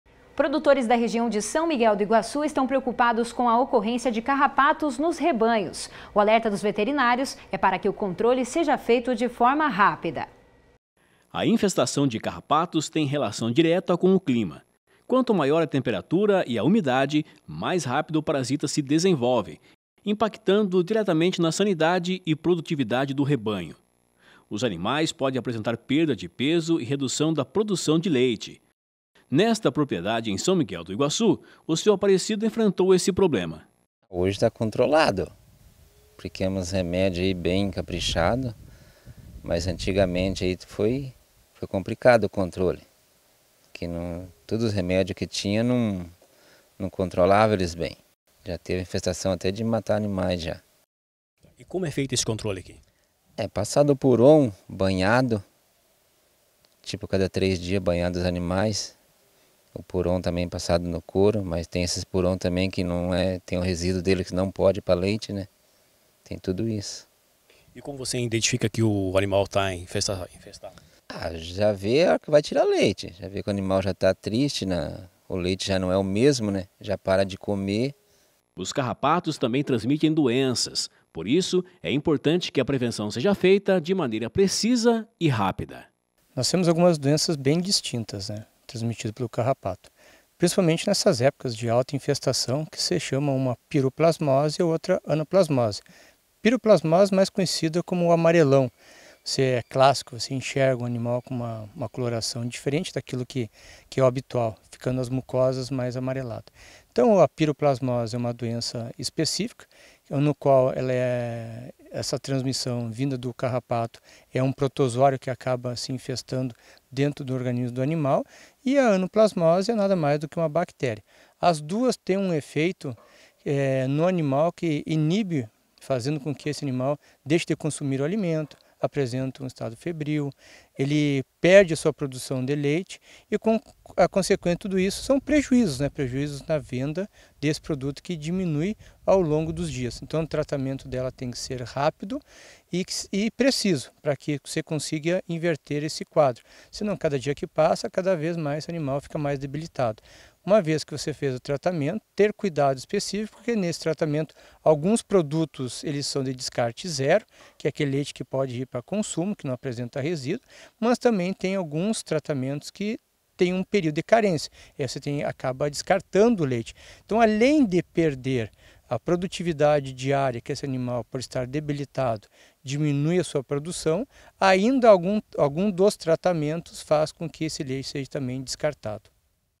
Agilidade é imprescindível para o controle de de carrapatos em bovinos
Produtores da região de São Miguel do Iguaçu (PR) estão preocupados com a ocorrência de carrapatos nos rebanhos. O alerta dos veterinários é para que o controle seja feito de forma imediata.

Participe conosco! Pode ser texto, áudio, fotos ou vídeos. Sugira assuntos para serem exibidos no programa.